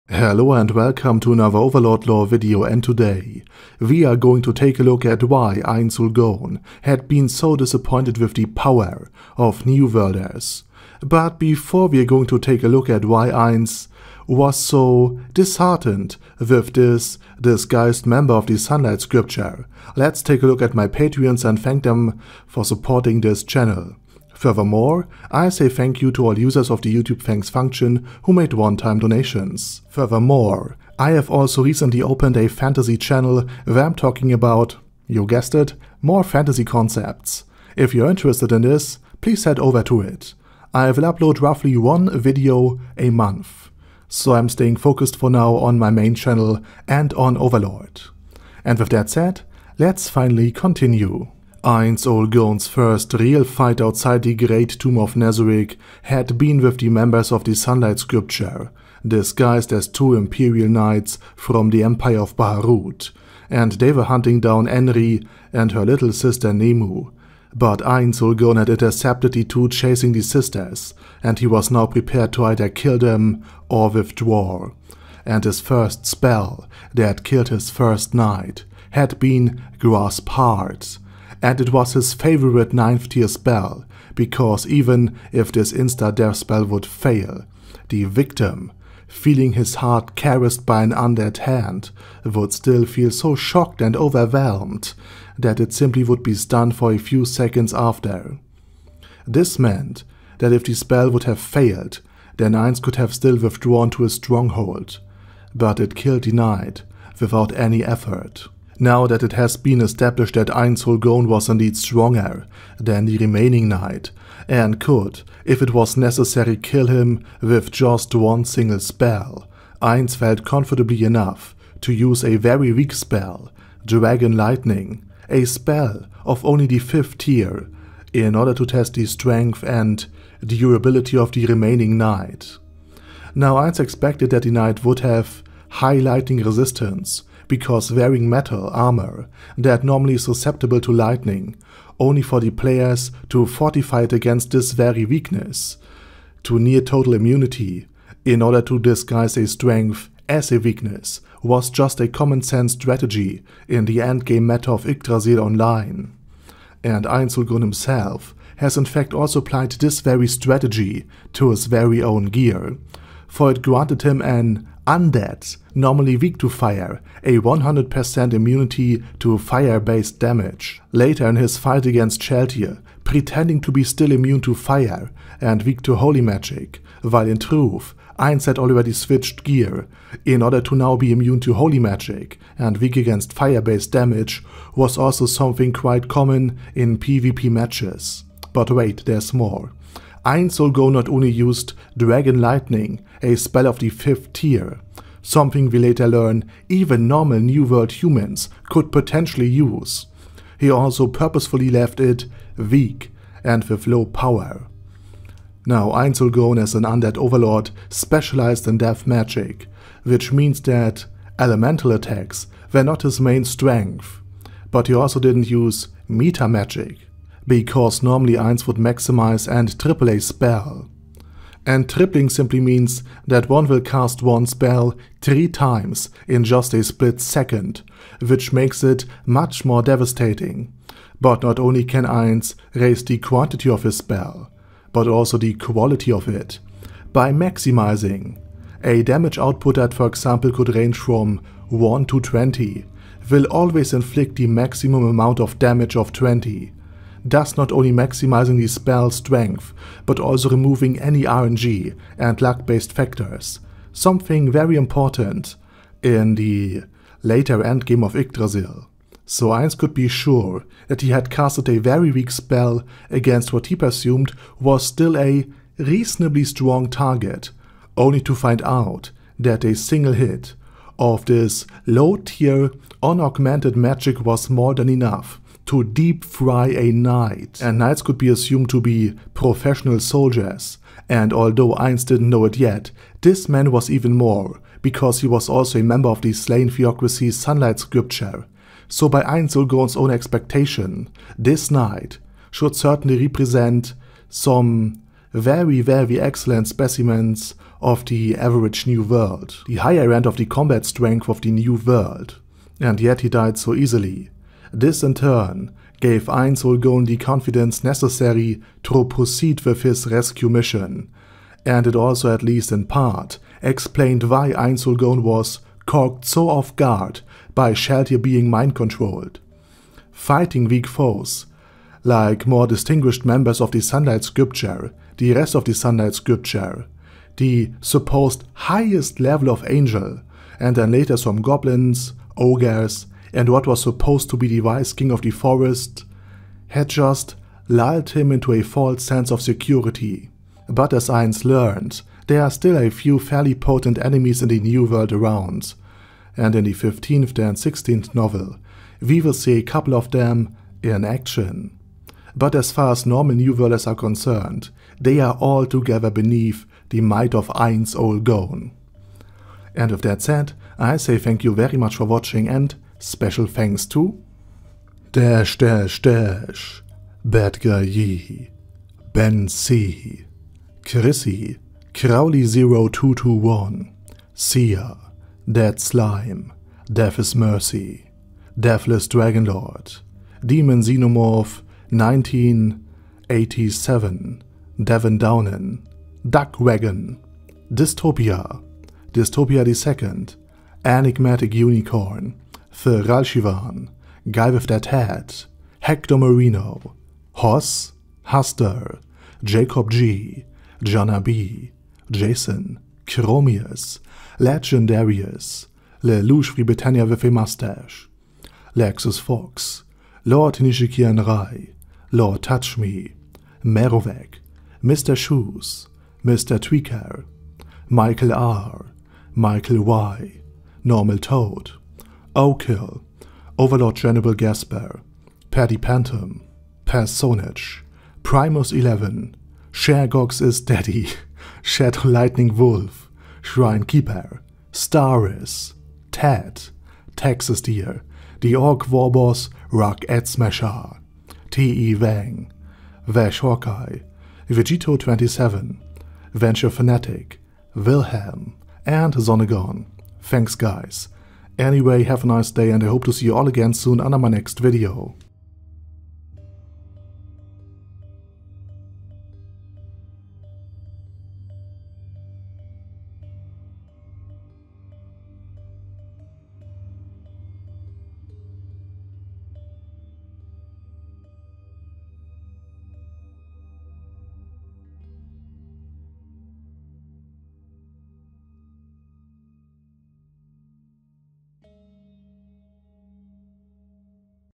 Why Ainz Ooal Gown was so disappointed with the Strenght of New Worlders
Ainz Ooal Gown came to the new world, initally fearing for his very life, not knowing how strong the rest of the world was, but what he found disappointed him, and ultimatly lulled him in a false sense of security.